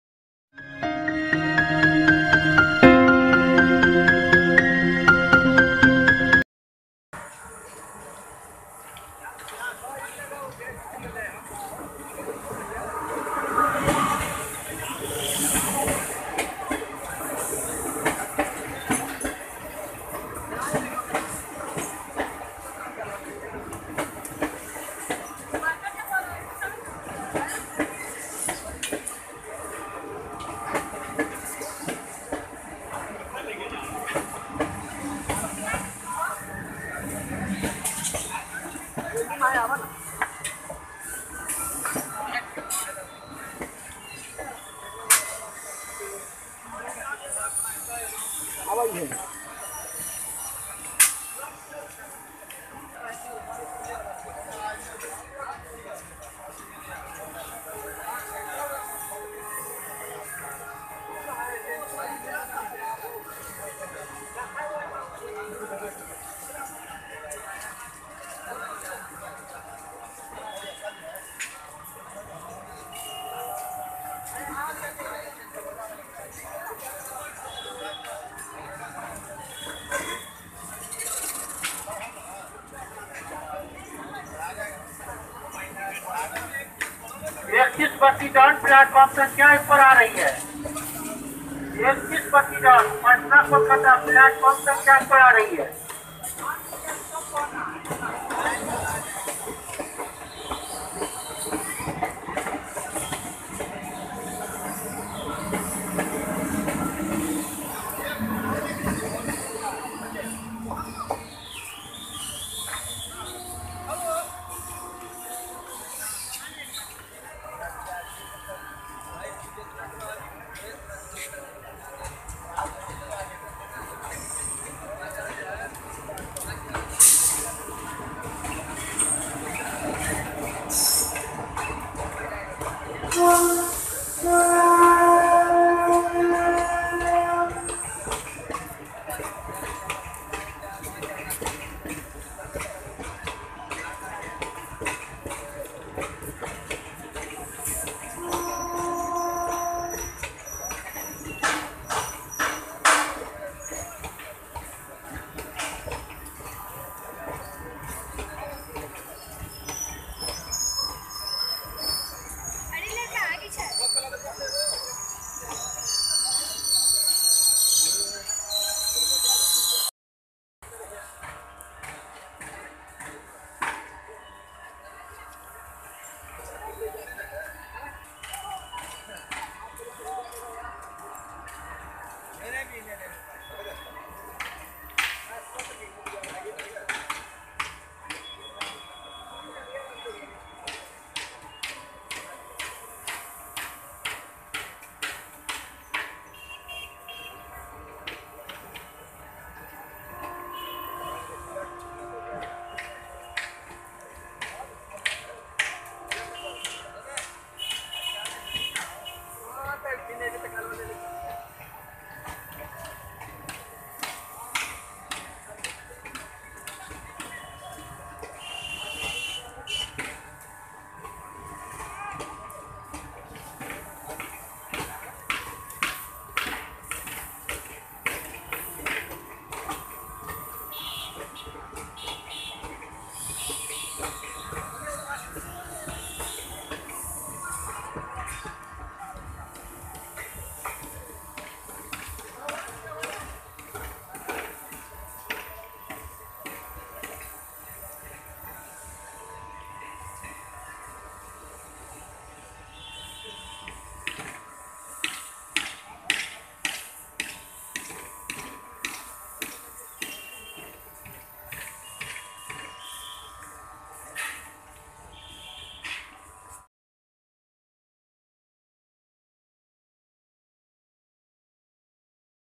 63261 फतुहा - बक्सर मेमू crossing 13132 पटना-कोलकाता एक्सप्रेस At Patna sahib
63261 फतुहा - बक्सर मेमू crossing 13132 पटना-कोलकाता एक्सप्रेस
At Patna sahib
13132 Patna - Kolkata Express
پٹنا کولکاتا ایکسپریس     पटना-कोलकाता एक्सप्रेस
Patna Junction -- Kolkata
Max Permissible Speed: 110 km hr between Jhajha and Barddhaman Junction
Type: Mail ,Express
Rake Zone: Eastern
Inaugural Run
Thu Jul 03, 2014
Pantry,Catering
✕ Pantry Car
✕ On-board Catering
✓ E-Catering
E-Catering available at PNBE, RJPB and KOAA
ICF Rake
63261 Fatuha - Buxar MEMU
فاتوہا -بکسر میمو     फतुहा - बक्सर मेमू
Fatuha Junction -- Buxar
Similar coach Composition for all MEMU running in DNR division and they all have RSA